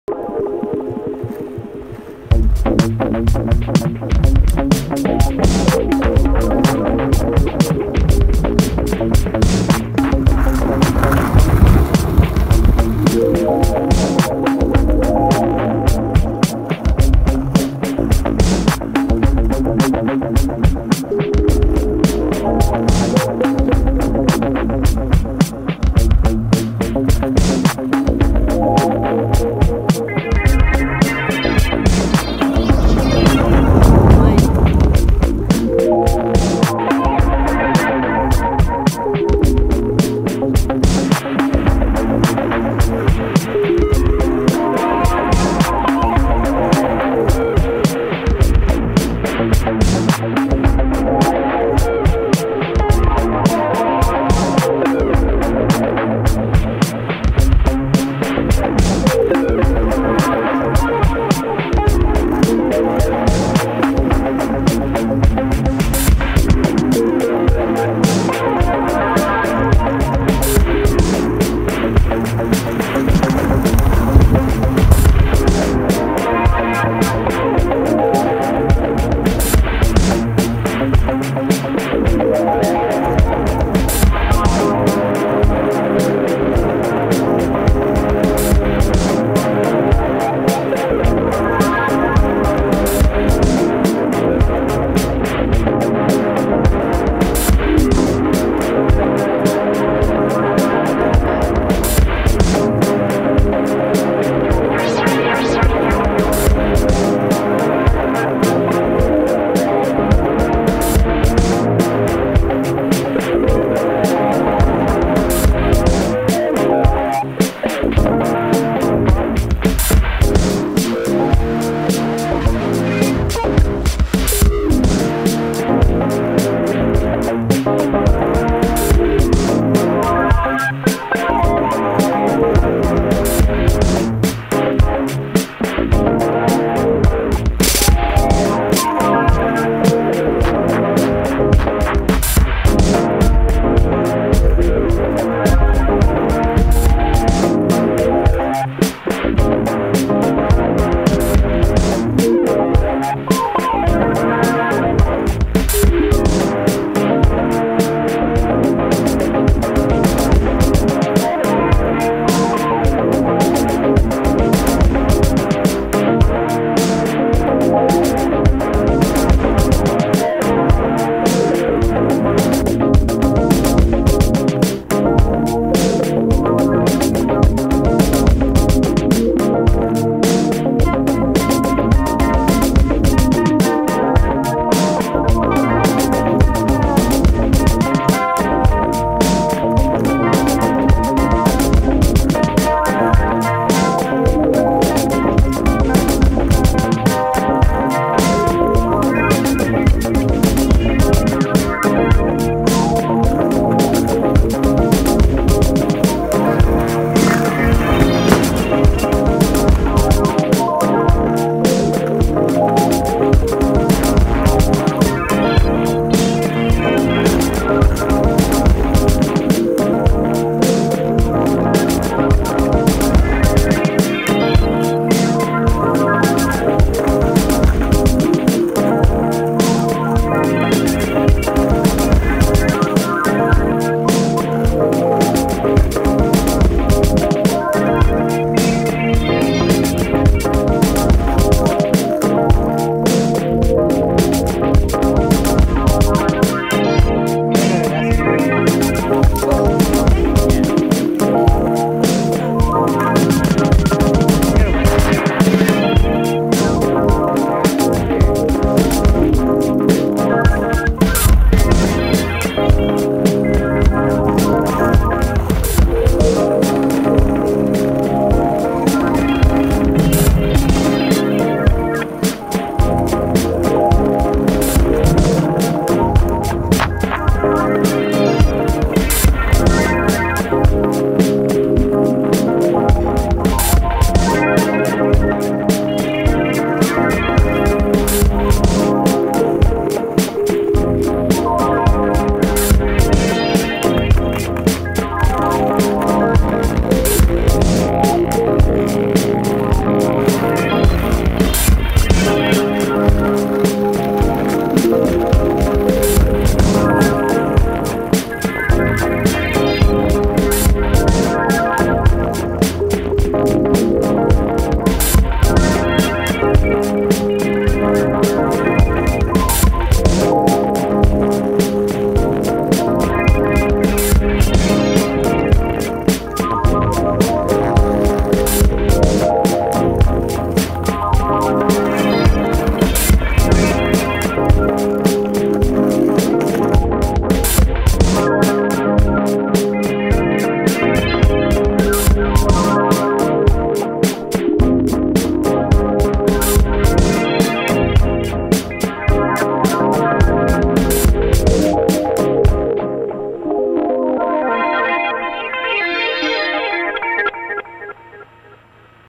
Skate Trance 4 - "Nat At Night"
Come aboard with us for a smooth cruise with fireworks and unicorns.
Inspirational Quote for Rail Trail - "There is no better place to push into trance"
Nat at Night is filmed on the Norwottuck Rail Trail in Hadley, MA.
Features friends old and older: A Juno 2, SH-101, JX-3p, Q80 Seq, DDD1, Ibanez multi fx, delay